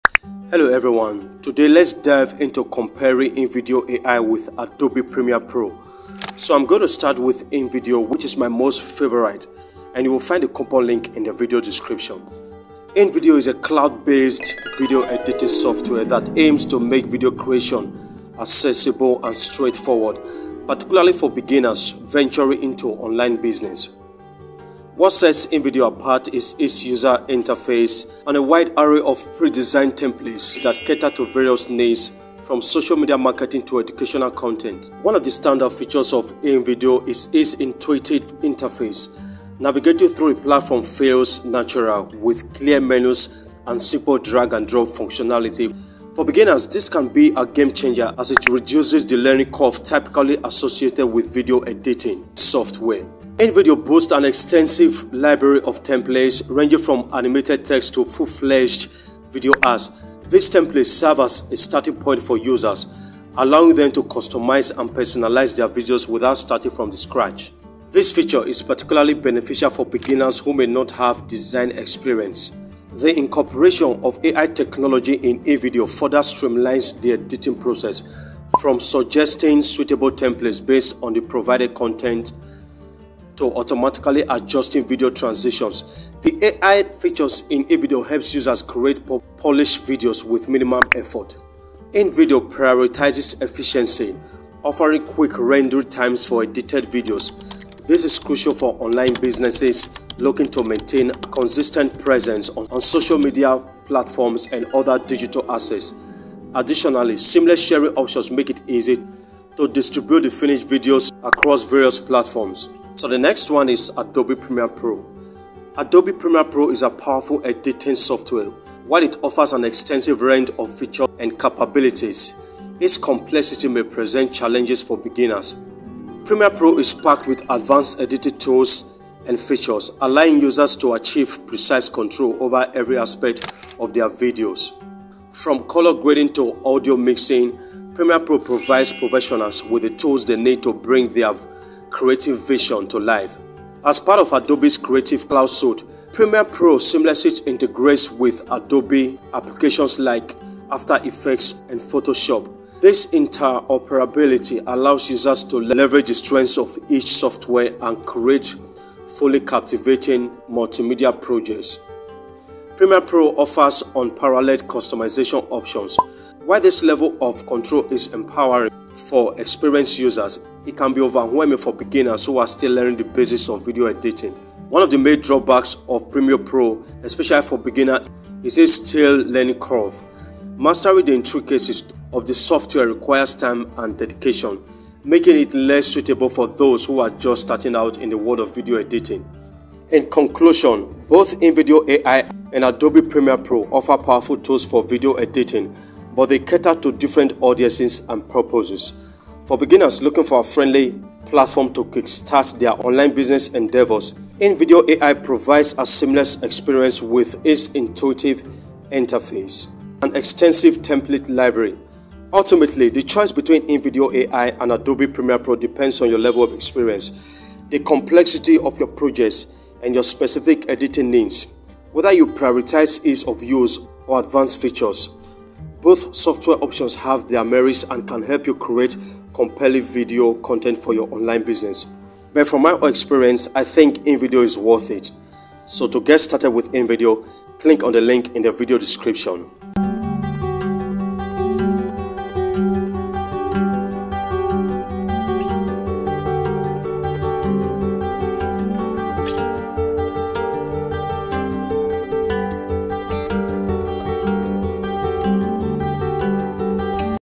Invideo And Adobe Premiere Pro | I tried them both
Invideo And Adobe Premiere Pro

In this video I will compare Invideo AI with Adobe Premiere Pro so that I will help you decide which one is best for you.
So you will learn in this video tutorial about Invideo vs Adobe premiere pro

image Source: Freepik

Invideo vs Adobe Premiere Pro